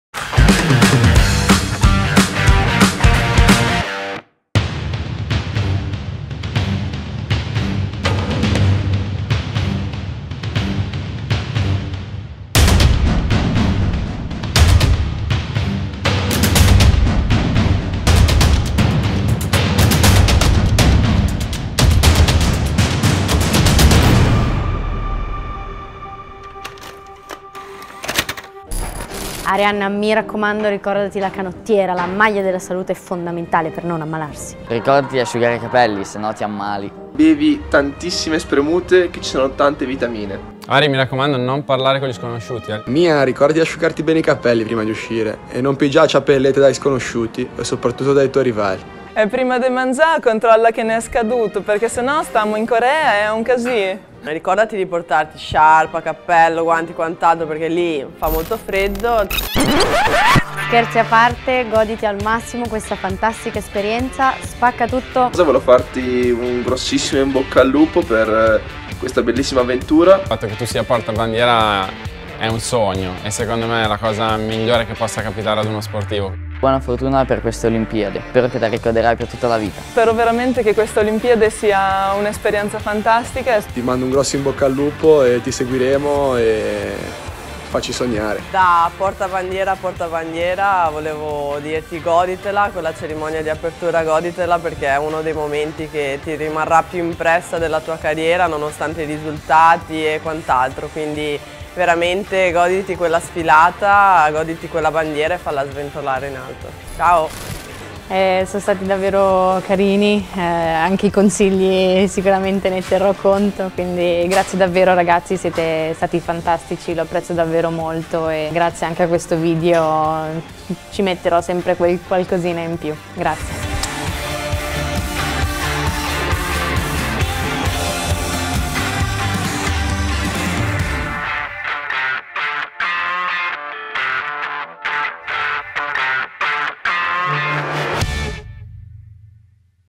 NextToPyeongChangUnipolSai | Raccomandazioni Arianna Fontana
Avrà messo tutto in valigia Arianna Fontana?
Seguirà tutte le raccomandazioni? Oltre a ricordarle di coprirsi bene e non accettare caramelle dagli sconosciuti i compagni del TeamYoungItalyUnipolSai hanno un messaggio molto speciale per lei.

Realizzato dall’agenzia di comunicazione Plug&Play